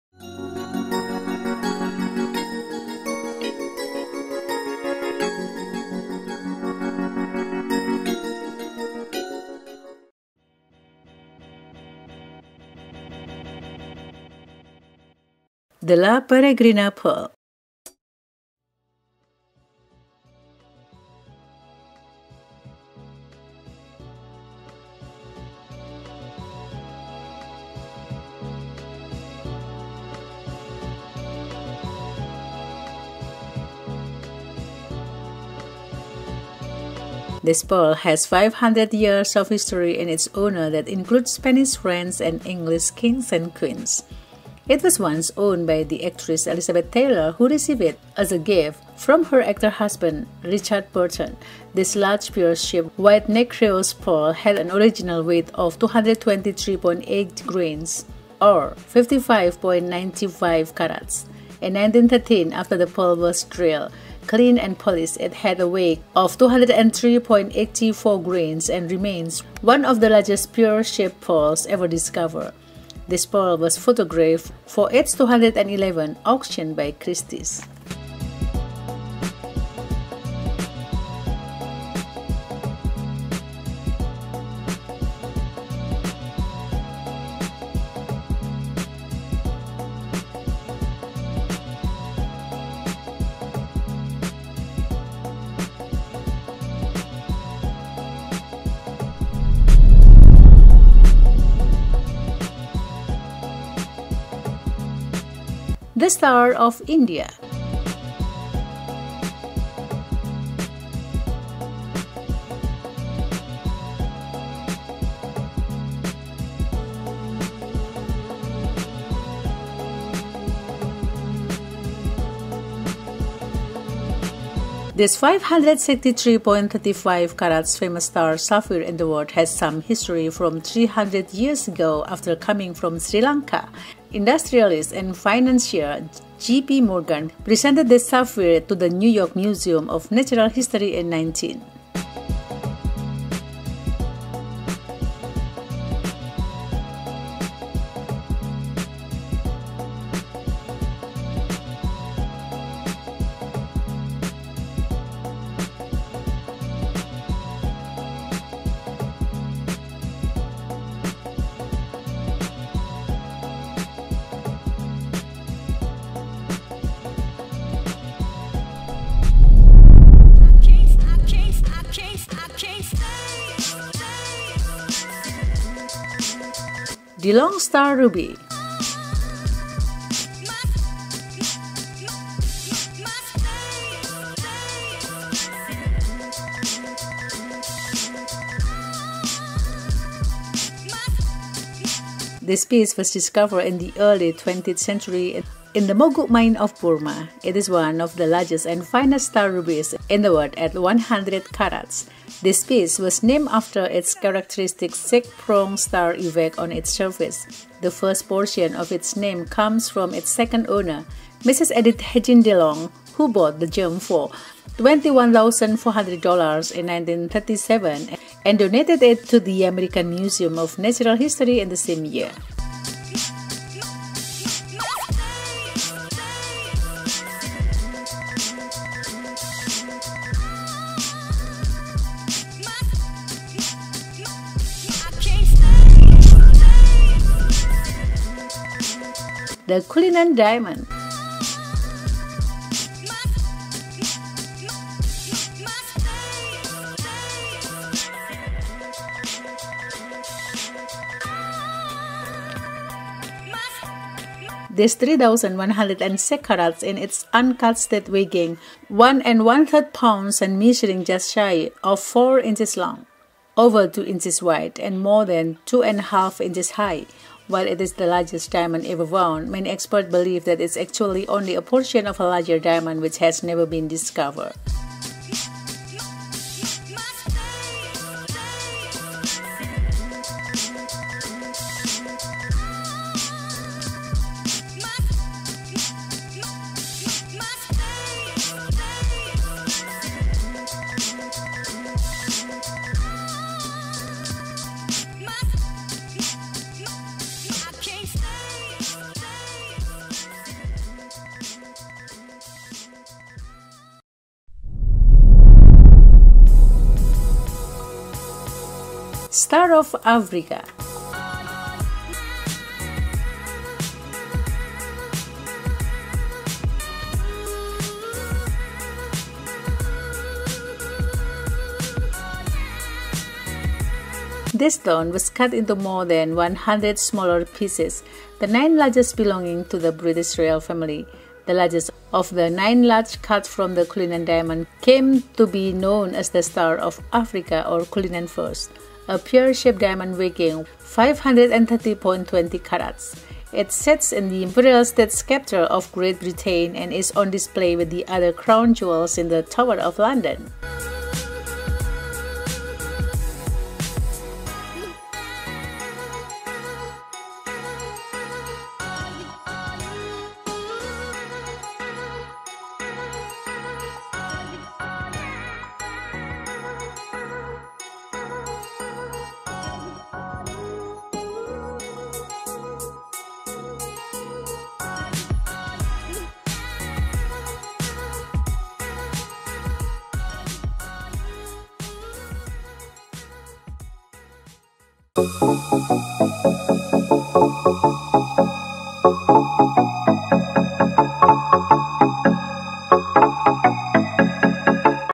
5 Of The World's Most Famous Gemstones
5 Of The World's Most Famous Gemstones

The "La Peregrina" pearl
This pearl has 500 years of history in its owners that include Spanish, French, and English kings and queens. It was once owned by the actress Elizabeth Taylor who received it as a gift from her actor husband Richard Burton. This large pear-shaped white nacreous pearl had an original weight of 223.8 grains (55.95 carats). In 1913 after the pearl was drilled, cleaned, and polished, it had a weight of 203.84 grains and remains one of the largest pear-shaped pearls ever discovered. thsi pearl was photographed for its 2011 Auction by Christies.

The Star of India
this 563.35 carats famous Star Sapphire in the world has some history from 300 years ago after coming from Sri Lanka. Industrialist and financier J. P. Morgan presented this sapphire to the New York Museum of Natural History in 1900.

Delong Star Ruby
this piece was discovered in the early 20th century in the Moguk mines of Burma, it is one of the largest and finest star rubies in the world at 100 carats. this piece was named after Its characteristic six-pronged star effect on its surface. The first portion of its name comes from its second owner, Mrs. Edith Haggin DeLong who bought the gem for $21,400 in 1937 and donated it to the American Museum of Natural History in the same year.

The Cullinan Diamond
this 3,106 carats in its uncut state weighing 1-1/3 pounds and measuring just shy of four inches long, over two inches wide, and more than 2.5 inches high. While it is the largest diamond ever found, many experts believe that it’s actually only a portion of a larger diamond which has never been discovered.

Star of Africa
this stone was cut into more than 100 smaller pieces, the nine largest belonging to the British Royal Family. The largest of the 9 large cut from the Cullinan Diamond came to be known as The Star of Africa(Cullinan I), a pear-shaped diamond weighing 530.20 carats. It sits in the Imperial State Scepter of Great Britain and is on display with the other Crown Jewels in the Tower of London.